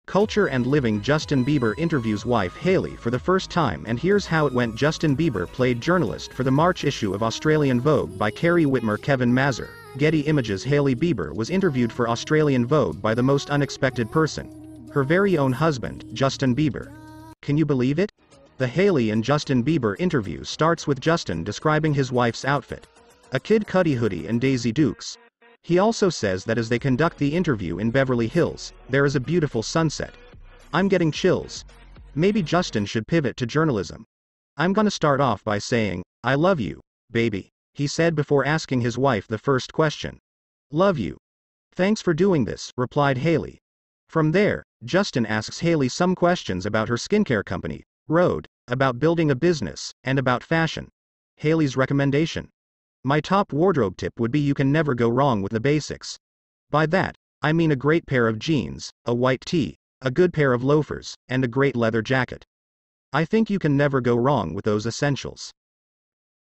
Justinbieber interviews wife first time
CULTURE & LIVING

Justin Bieber interviews wife Hailey for the first time and here's how it went

Justin Bieber played journalist for the March issue of Australian Vogue

BY CARRIE WITTMER

1 March 2023

Kevin Mazur/Getty Images

Hailey Bieber was interviewed for Australian Vogue by the most unexpected person: her very own husband, Justin Bieber. Can you believe it? The Hailey and Justin Bieber interview starts with Justin describing his wife’s outfit: a Kid Cudi hoodie and Daisy Dukes. He also says that as they conduct the interview in Beverly Hills, there is a beautiful sunset. I’m getting chills. Maybe Justin should pivot to journalism? “I’m gonna start off by saying, I love you, baby,” he said before asking his wife the first question.Love you. Thanks for doing this,” replied Hailey.

From there, Justin asks Hailey some questions about her skincare company, Rhode, about building a business, and about fashion. Hailey’s recommendation? “My top wardrobe tip would be you can never go wrong with the basics. By that, I mean a great pair of jeans, a white tee, a good pair of loafers, and a great leather jacket. I think you can never go wrong with those essentials.”